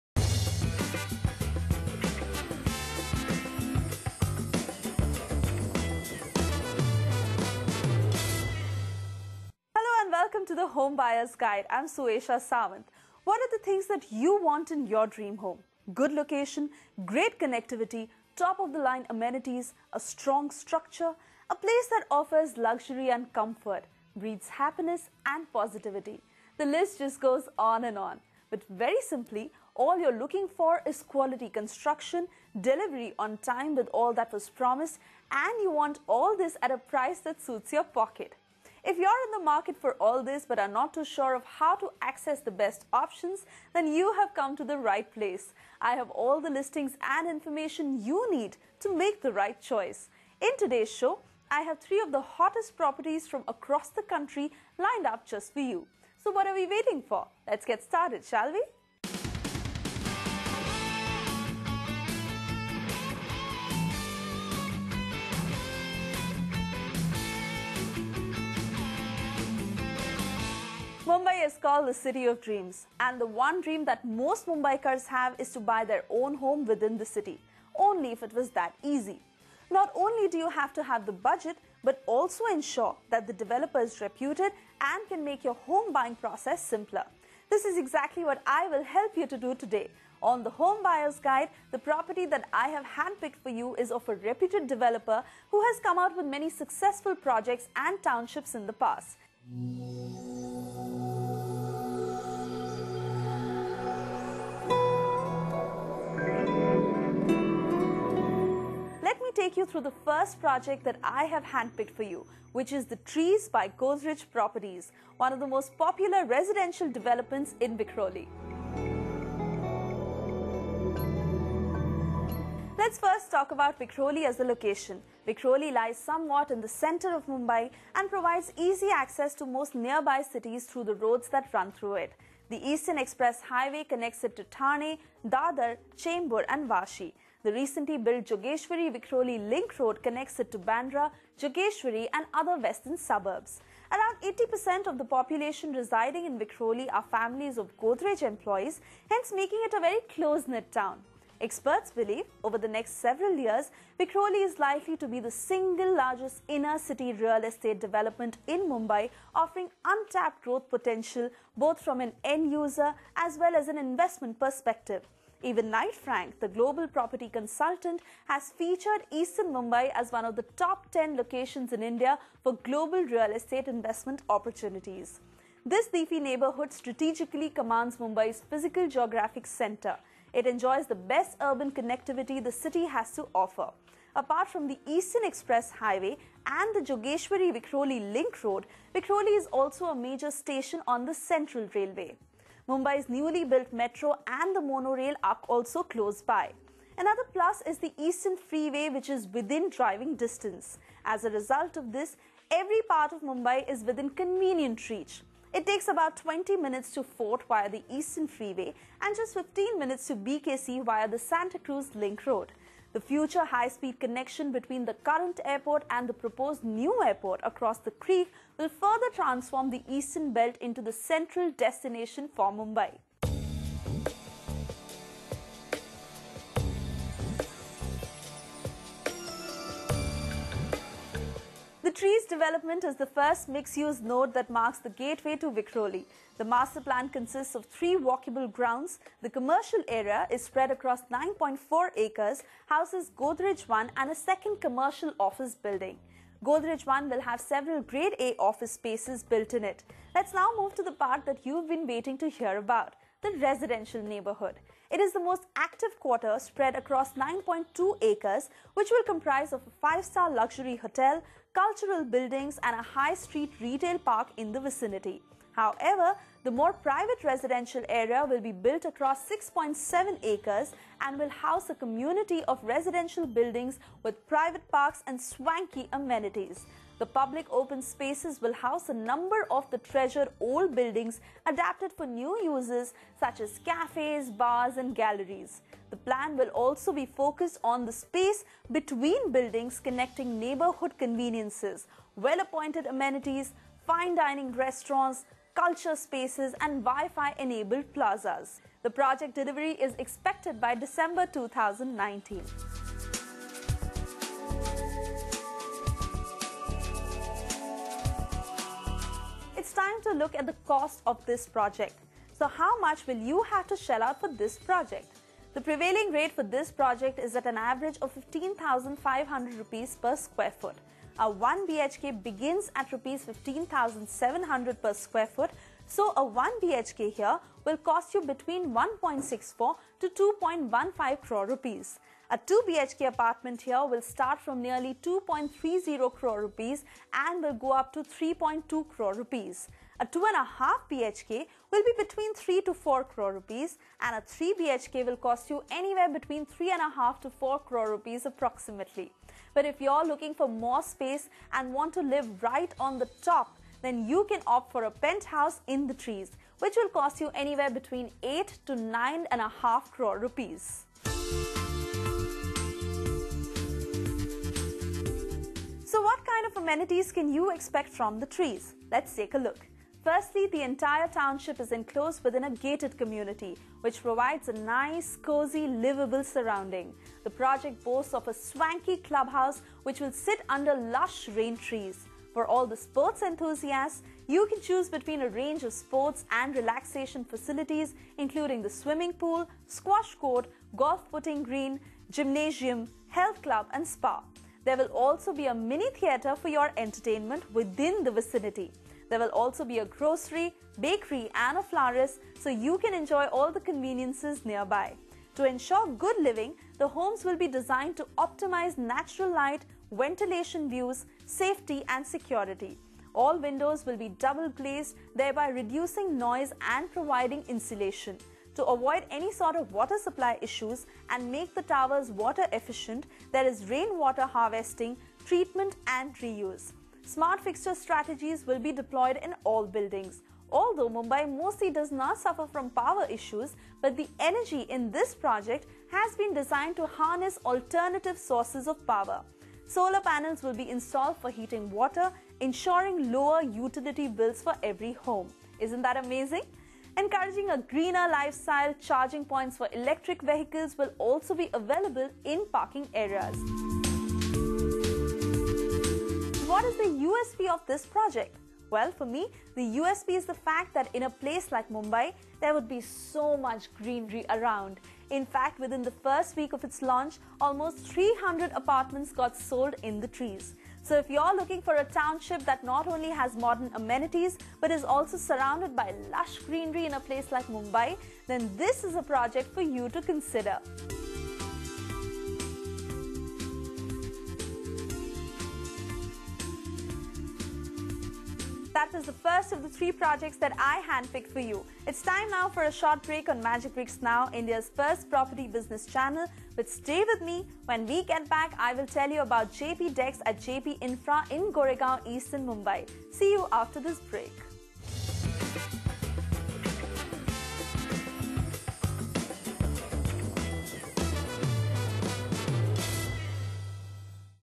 The Home Buyers Guide – The Trees by Godrej Properties
The Home Buyer’s guide is an armchair guide to some of the hottest properties in the market today. A well-connected city is always a dream for any home buyer. Magicbricks Now takes an inside look at The Trees by Godrej Properties. Tune in to the video to know more: